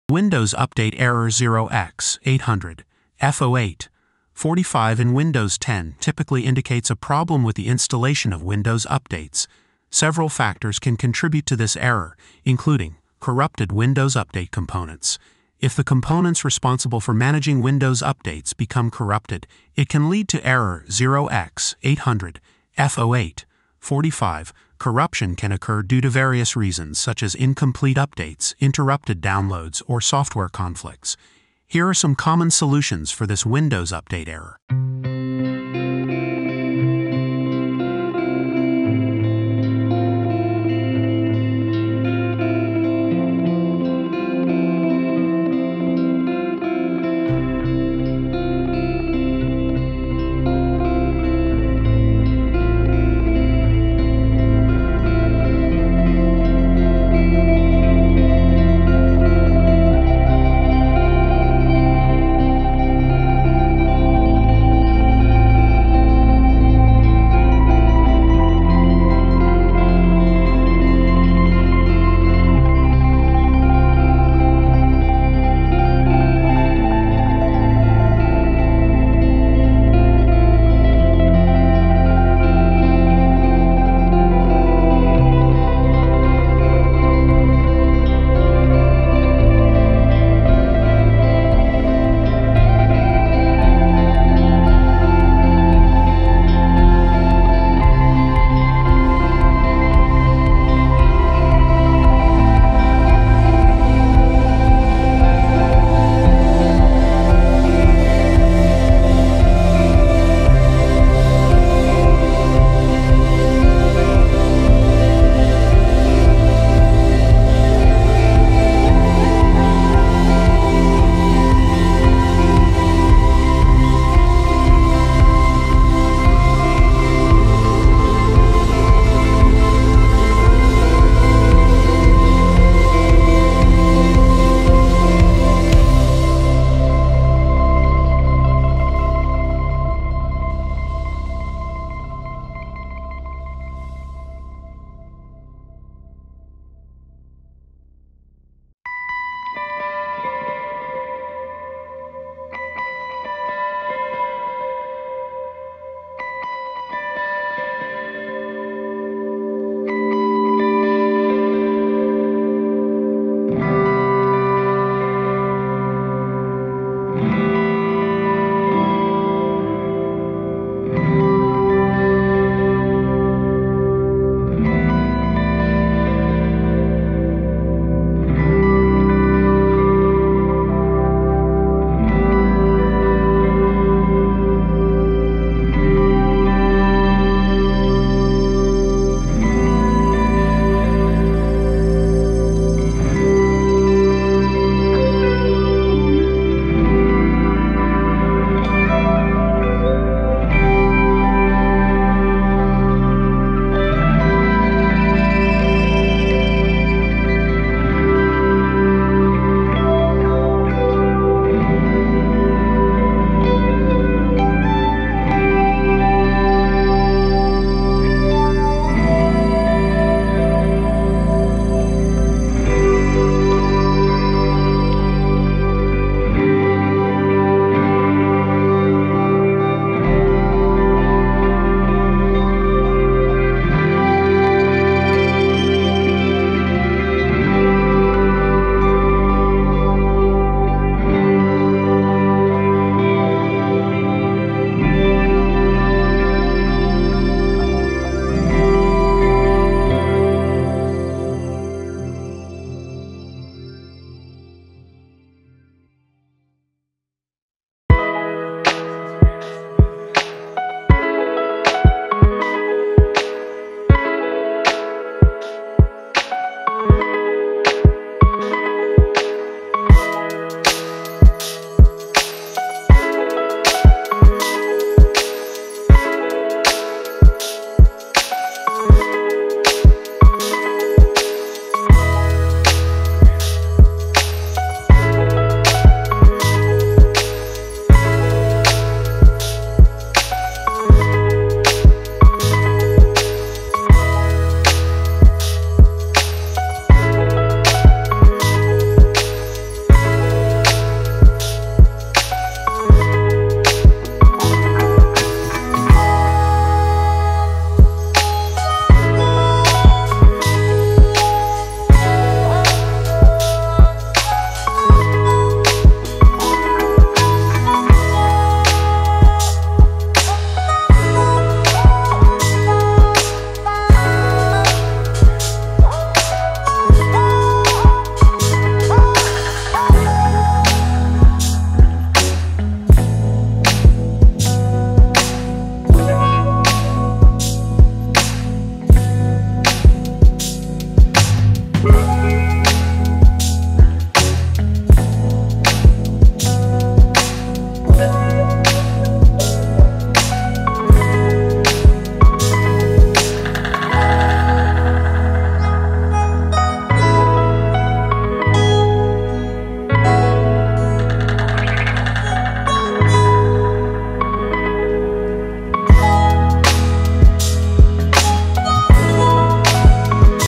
Windows 10 Update Error Code 0x800f0845 There Were Some Problems Installing Updates FIX [2026]
The Windows Update error 0x800f0845 in Windows 10 typically indicates a problem with the installation of Windows updates. Several factors can contribute to this error, including:
Corrupted Windows Update components: If the components responsible for managing Windows updates become corrupted, it can lead to error 0x800f0845. Corruption can occur due to various reasons, such as incomplete updates, interrupted downloads, or software conflicts. Here are some common solutions for this Windows Update Error .

 》 CHAPTERS 《

00:00 Intro
00:35 Solution 1
02:12 Solution 2
04:21 Solution 3
06:03 Outro